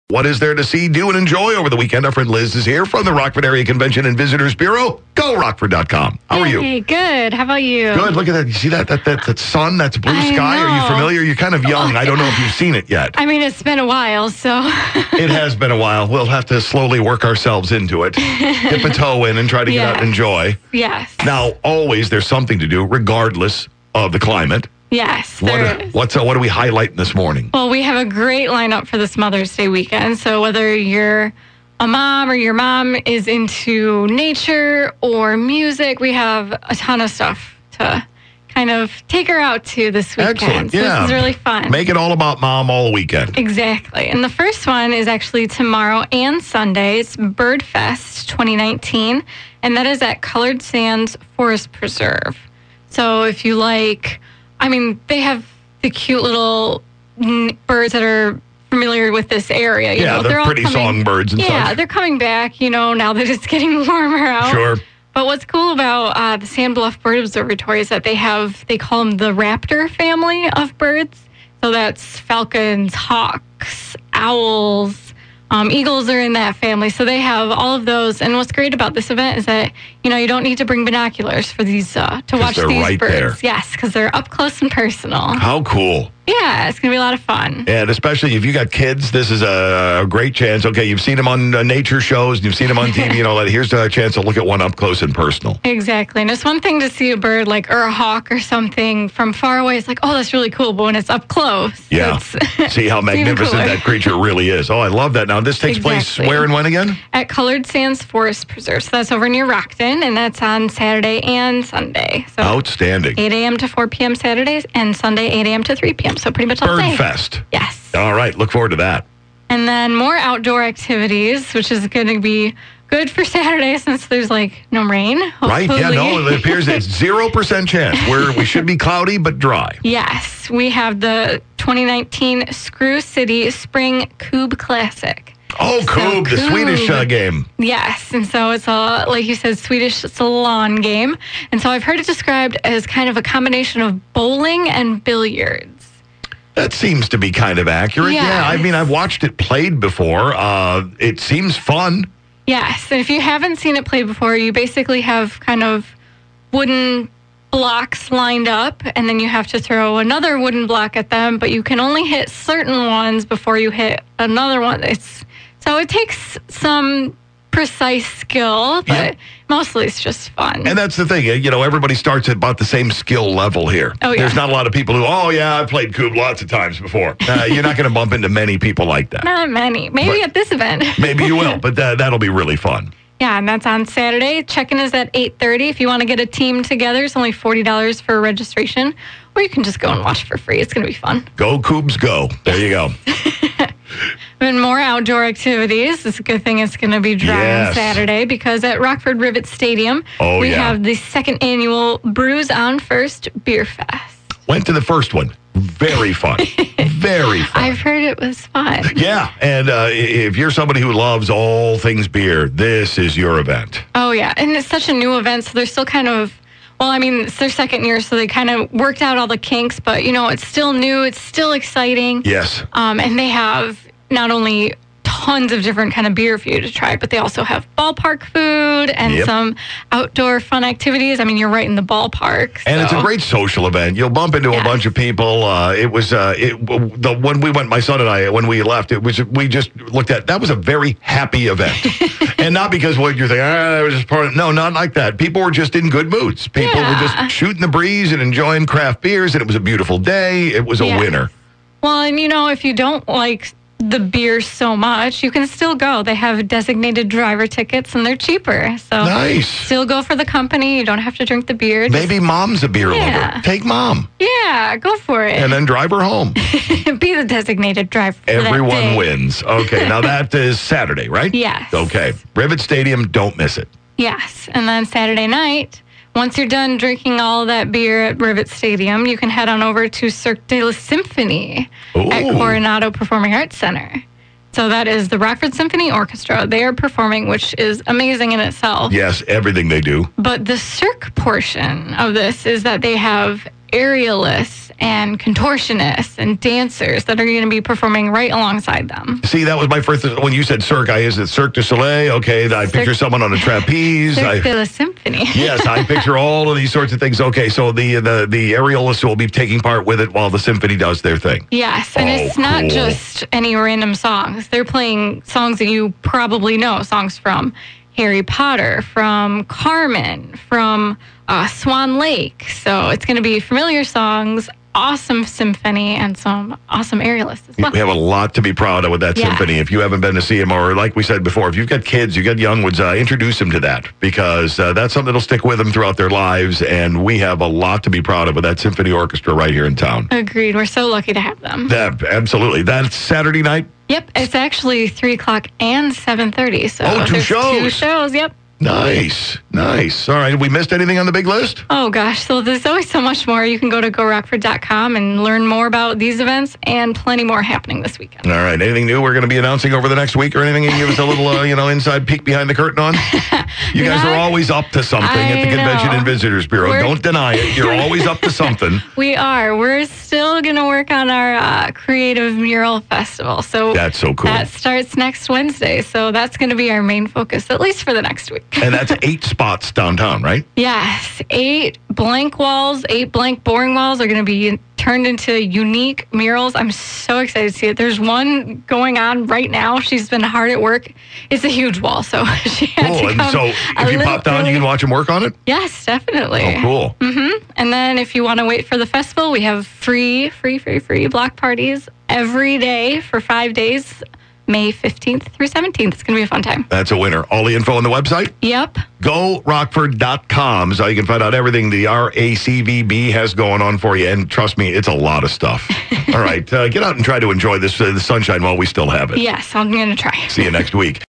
Mother's Day Activities In Rockford
Liz from GoRockford.com made her weekly visit to the show to let us know what to do over the weekend.